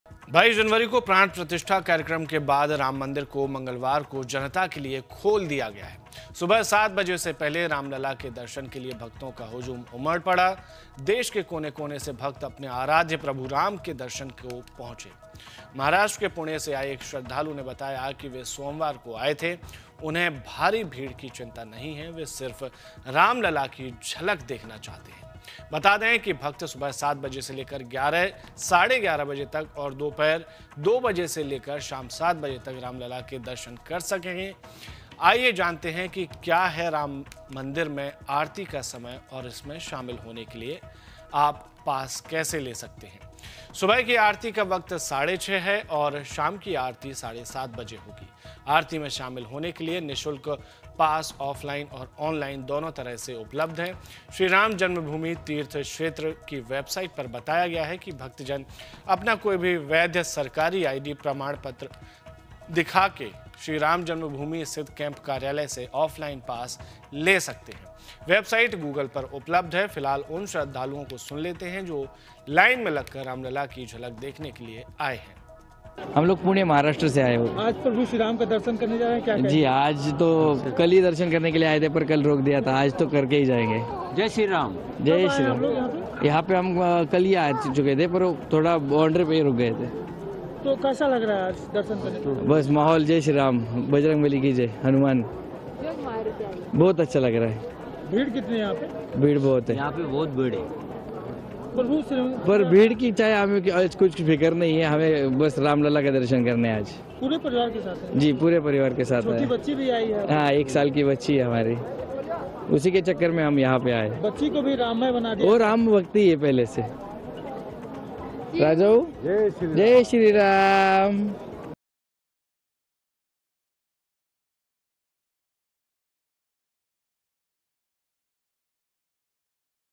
Ram Mandir Ayodhya: दर्शन करने पहुंचे श्रद्धालुओं ने व्यवस्था को लेकर क्या कहा? | Pran Pratishtha
22 जनवरी को प्राण प्रतिष्‍ठा कार्यक्रम के बाद राम मंदिर को मंगलवार को जनता के लिए खोल दिया गया है। सुबह सात बजे से पहले रामलला के दर्शन के लिए भक्‍तों का हुजूम उमड़ पड़ा। देश के कोने-कोने भक्‍त अपने आराध्‍य प्रभु राम के दर्शन को पहुंचे। महाराष्ट्र के पुणे से आए एक श्रद्धालु ने बताया कि वे सोमवार को आए थे। उन्‍हें भारी भीड़ की चिंता नहीं है, वे सिर्फ राम लला के दर्शन करना चाहते हैं। बता दें, भक्‍त सुबह सात बजे से लेकर 11:30 बजे तक और दोपहर दो बजे से लेकर शाम सात बजे तक रामलला के दर्शन कर सकेंगे।